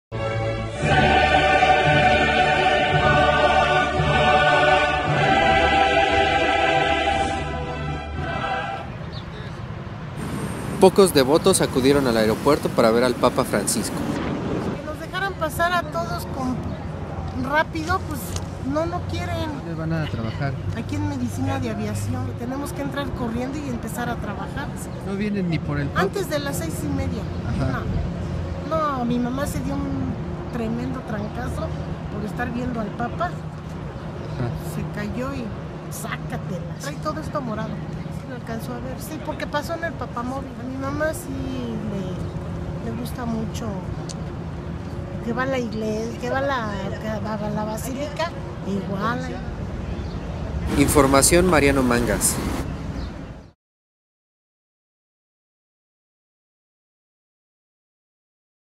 PapaEnMex Feligreses fueron menos al AICM
Pocos devotos acudieron al aeropuerto para ver al papa Francisco.